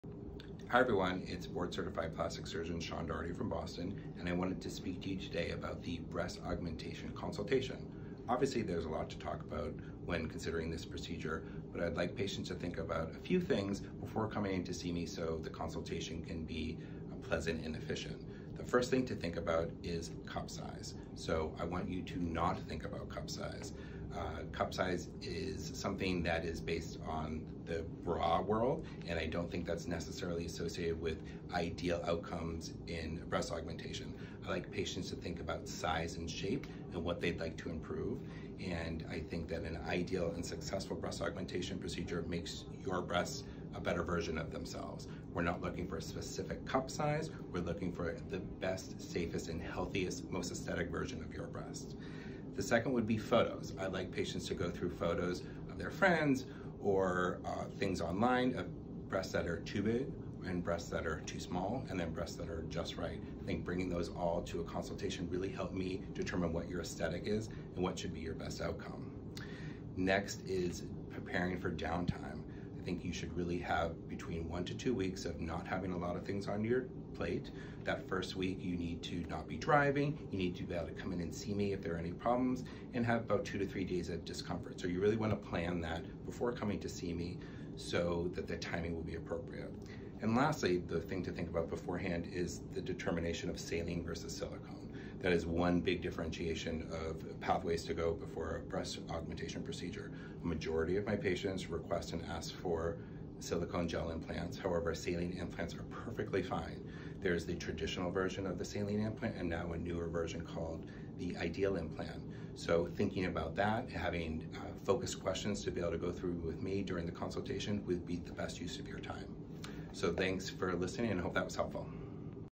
Tips For Your Breast Augmentation Consultation!
Dr. Sean Doherty shares his best tips for you during your Breast Augmentation consultation.

69 Newbury,5th Floor
Boston, MA 02116

1 Brookline Place, Suite 427
Brookline, MA 02445